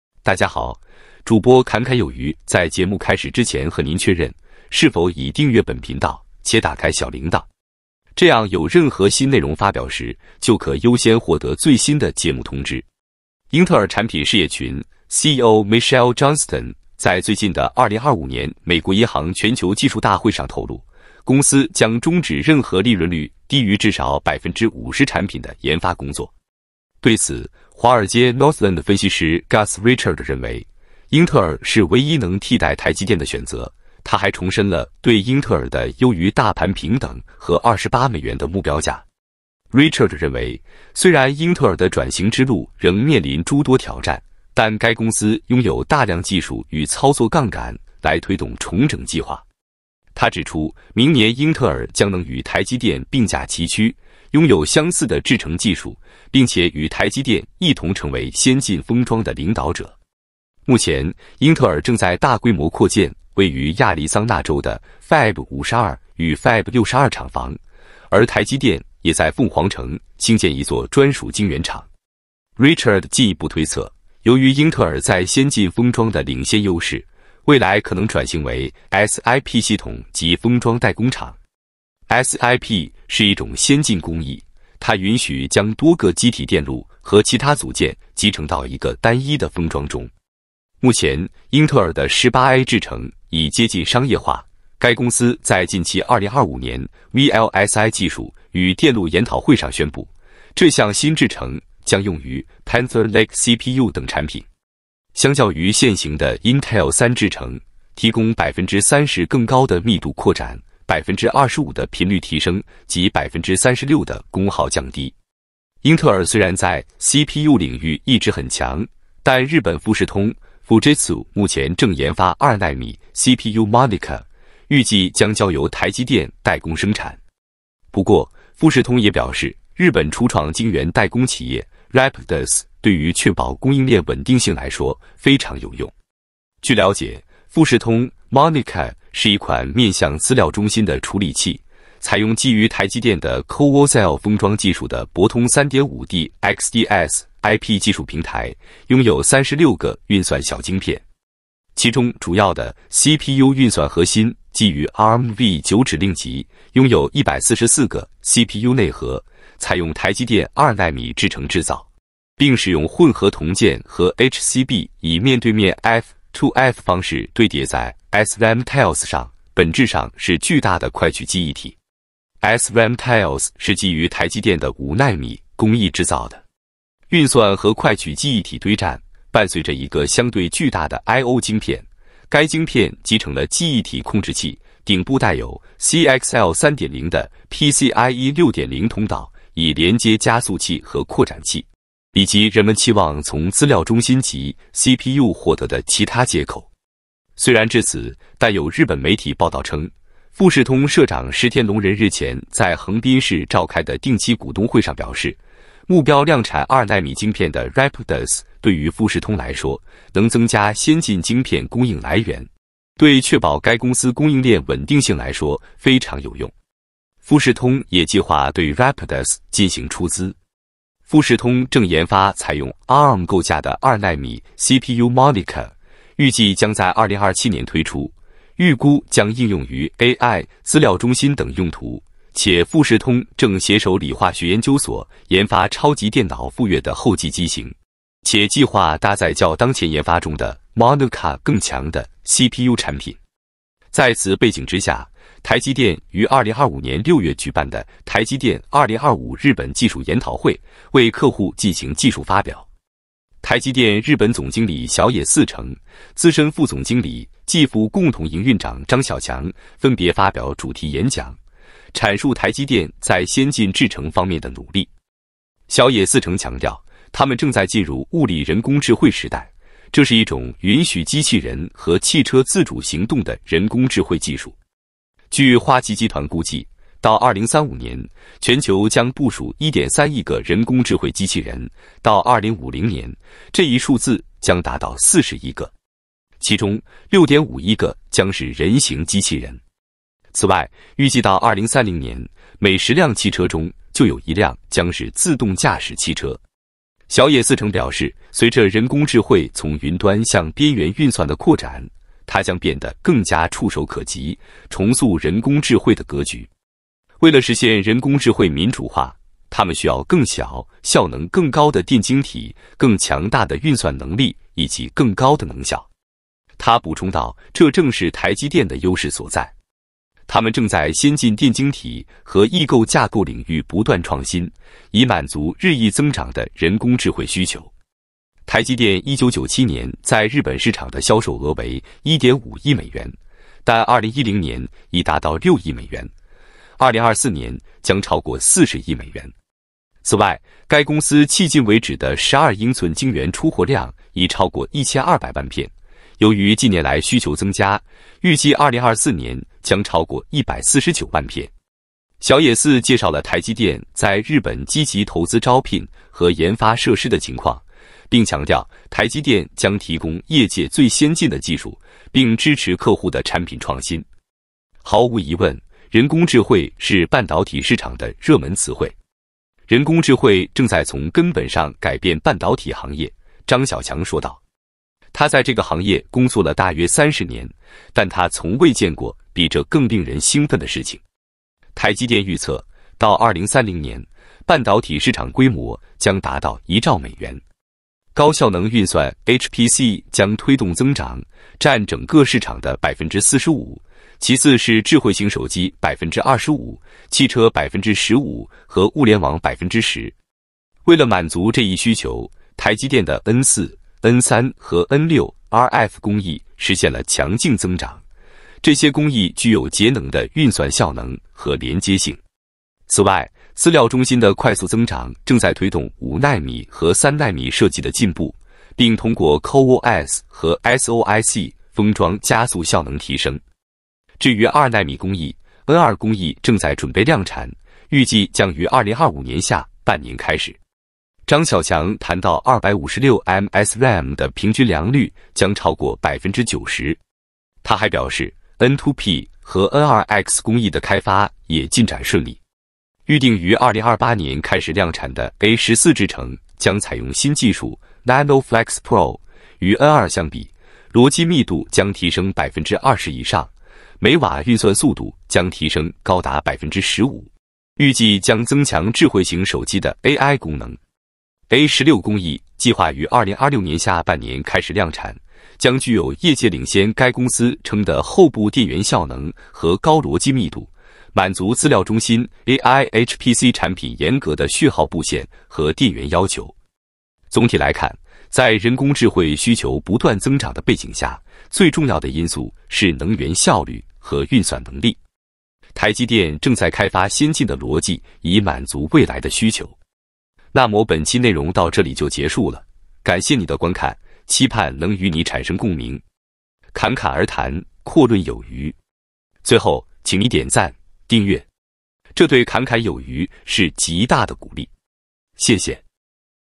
暗藏玄機！台積電2奈米SRAM良率超90%背後極不簡單，富士通晶片嚇人。改寫資料中心級CPU歷史，英特爾狂顫抖分析師仍看好。CoWoS-L封裝技術加持，博通3.5D XDSiP技術平台顯神通。
目前英特爾的18A制程已接近商業化，該公司在近期2025年VLSI技術與電路研討會上宣佈，這項新制程將用於Panther Lake CPU等產品。富士通Monaka是一款面向資料中心的處理器，採用基於台積電的CoWoS-L封裝技術的博通3.5D XDSiP技術平台，擁有36個運算小晶片。其中主要的CPU運算核心基於Armv9指令集，擁有144個CPU內核，採用台積電2nm制程製造。人工智慧是半導體市場的熱門詞彙。人工智慧正在從根本上改變半導體行業，張曉強說道。他在這個行業工作了大約30年，但他從未見過比這更令人興奮的事情。